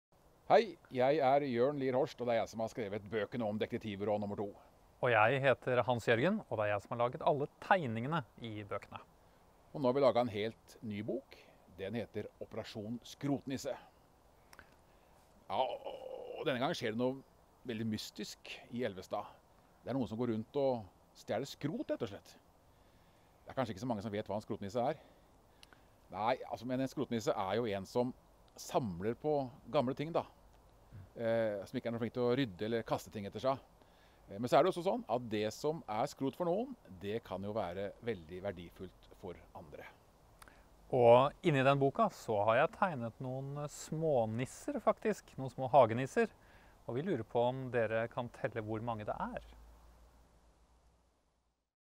Ukens forfatter: Jørn Lier Horst og illustratør Hans Jørgen Sandnes
Jørn Lier Horst og Hans Jørgen Sandnes er ute med en ny bok om Detektivbyrå nr. 2. Hva er egentlig en skrotnisse?